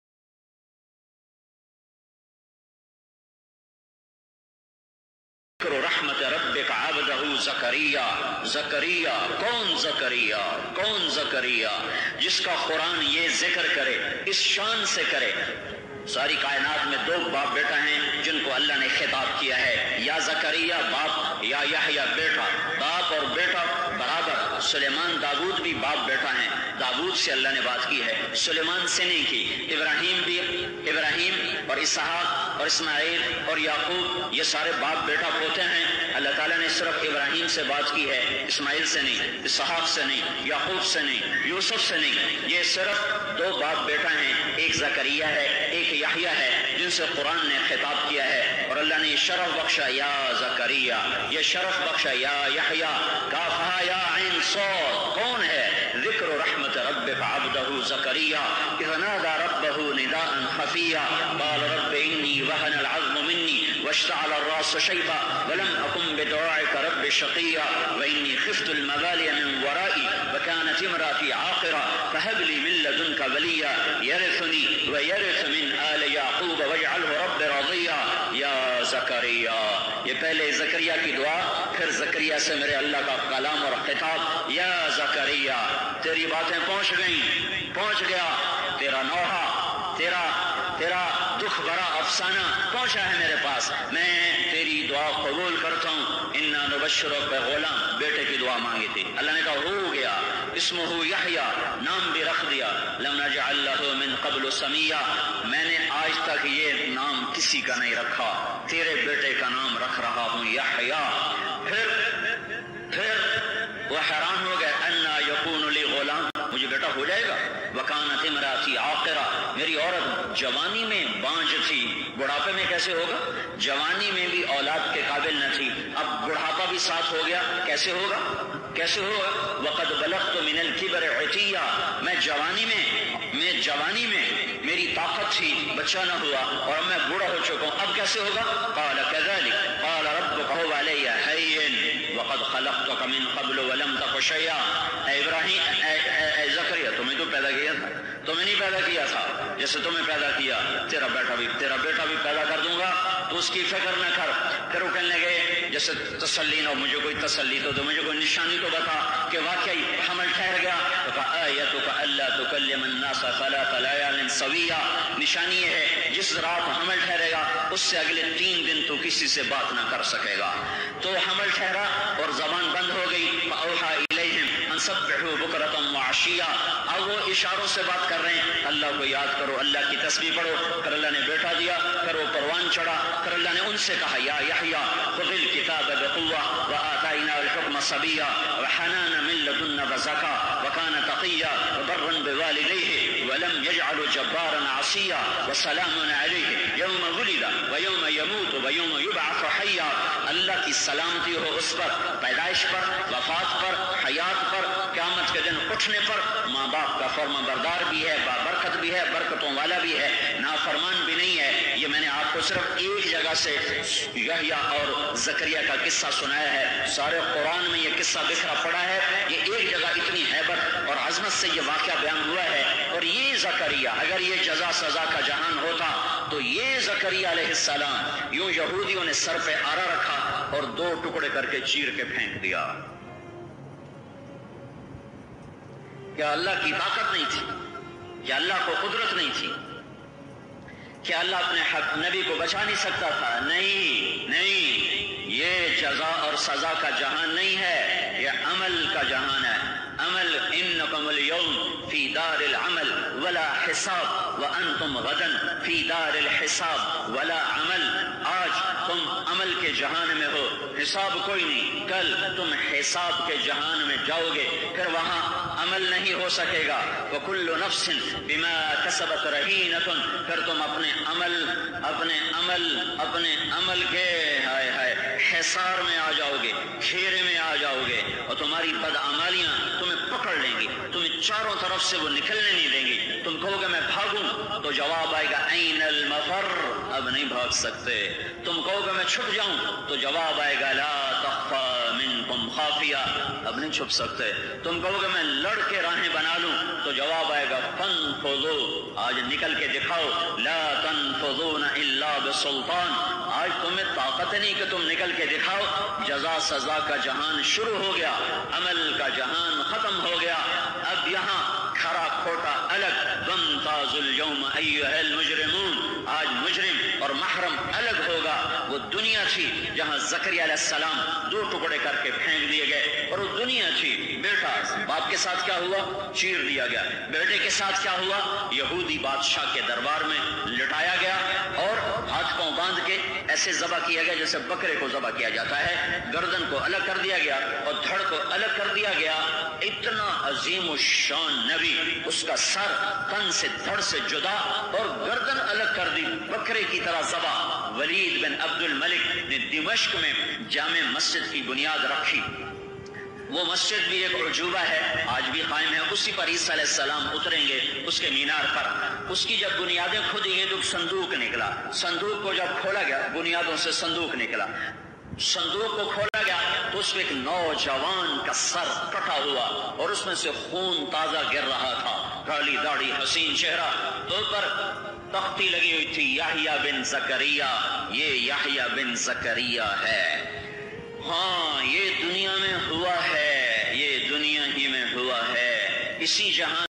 Hazrat Yahya(A. S) ko yahudyon ne aare se kiun kata..|maulana tariq jameel bayan emotional 2020
حضرت زکریا علیہ السلام اور حضرت یحییٰ علیہ السلام دونوں باپ بیٹا جن سے اللہ نے براہ راست بات کی... حضرت یحییٰ علیہ السلام کو یہودیوں نے آرے سے چیر دیا اور دو ٹکڑے کر دیے. اور ان کے بیٹے یحیی علیہ السلام کو یہودیوں کے بادشاہ کے دربار میں لا کر ذبح کر دیا گیا سر تن سے جدا کر دیا. ولید بن عبدالملک نے دمشق میں جامع مسجد کی بنیاد رکھی جس پر حضرت عیسی علیہ السلام علیکم گے اس کی بنیادیں کھوکھلی گئی تو اس نے ایک صندوق ملا جس میں ایک نوجوان کا سر پڑھا تھا کالی داڑھی حسین چہرہ اور اس کے ساتھ ایک تختی پڑی تھی جس پر لکھا تھا یحییٰ بن زکریا.
maulana tariq jameel bayan emotional 2020
in this video we will talk about hazrat yahya(A.S)